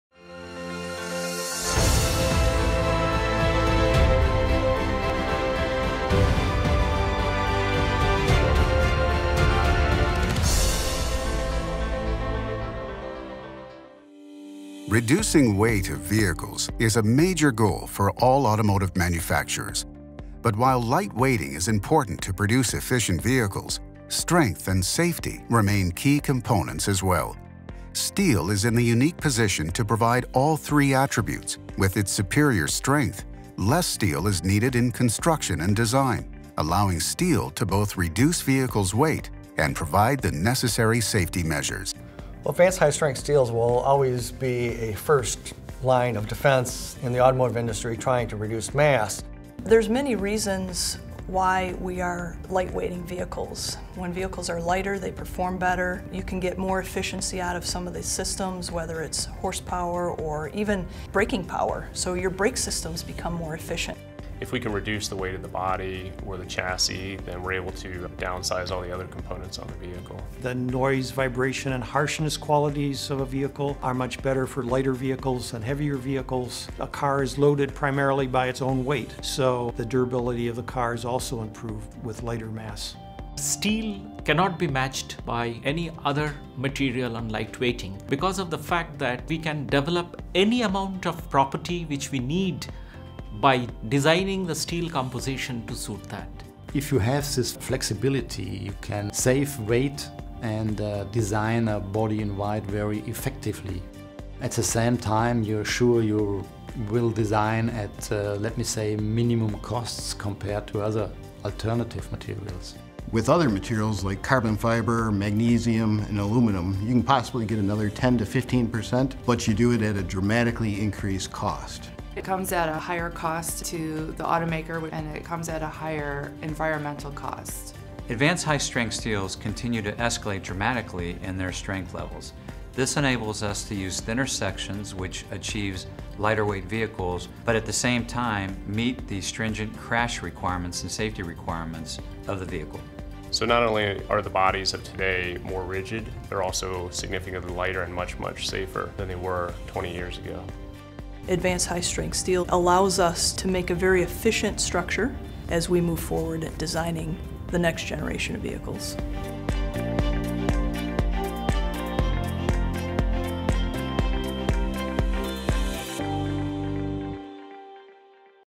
STEEL YOUR WEIGHT
Advanced High-Strength Steels is the new vehicle lightweighting material. Listen to automakers, academics and steel engineers explain why it's the fastest growing material in automotive.